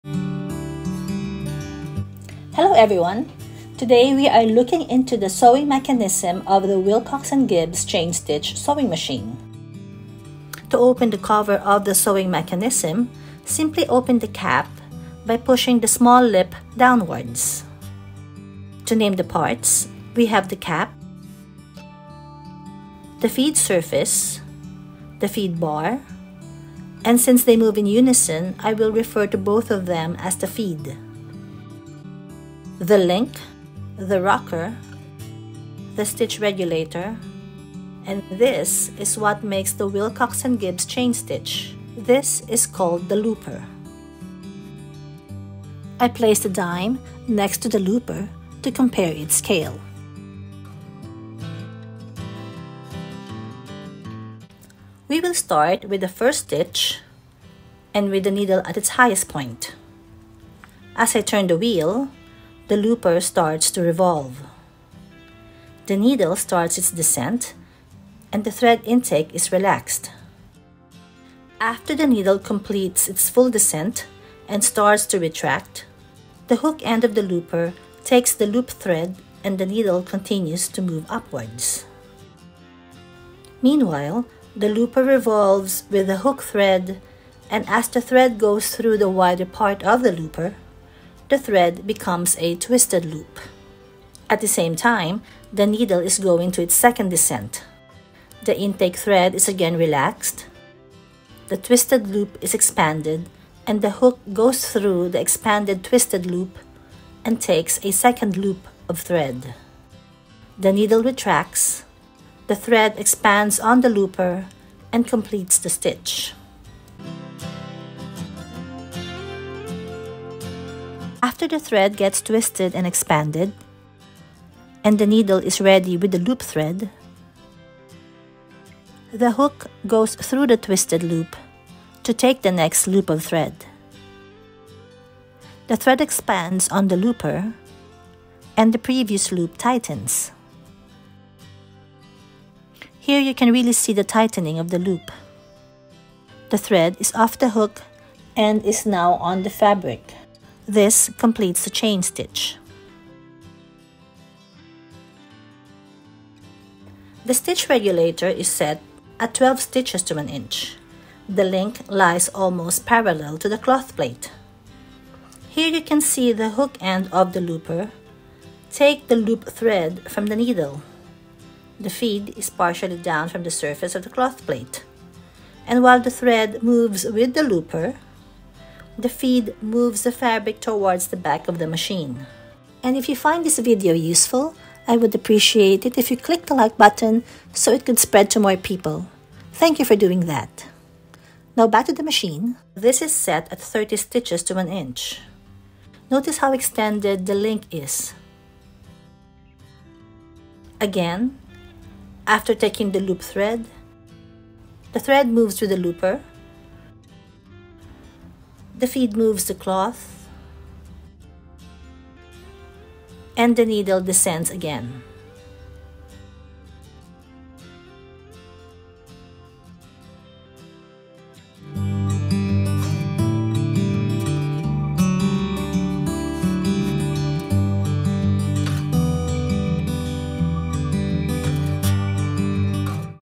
How Stitches are Formed on a Willcox & Gibbs Sewing Machine
Ever wonder about the Willcox & Gibbs sewing chain stitches?

Timeline
00:00 A Closer Look
00:12 Inside the Cap
00:52 Let's Break It Down
01:58 An Even Closer Look
02:37 Feed Movement